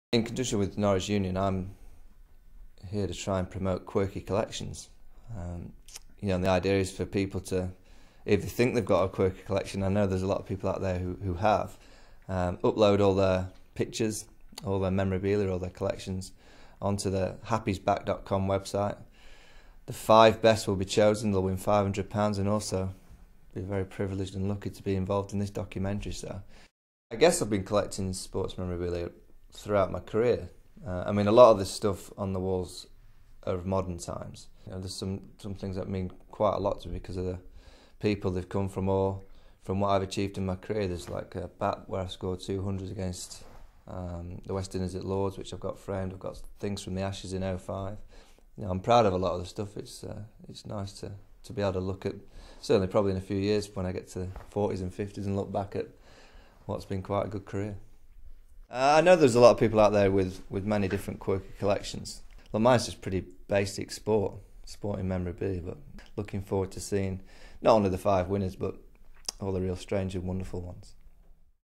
Michael Vaughan Reveals his Quirky Collection
Norwich Union is set to get curtains twitching by throwing open the doors to some of the UKs most weird and wonderful collections as it launches a search for closet collectors to come forwards and star in a new film.

The UKs largest insurer is seeking images and anecdotes of the countrys quirkiest collections to be uploaded onto the best will feature in a short online movie celebrating eccentric collectors.

And to ensure the best entrants have extra cash in their wallets to pursue their passion, Norwich Union, soon to be Aviva, is giving the five winners £500 each. It is also offering free contents insurance to new customers when they buy buildings insurance.

So if youre mad about memorabilia, crazy about comics or barmy about beer mats this is the competition for you!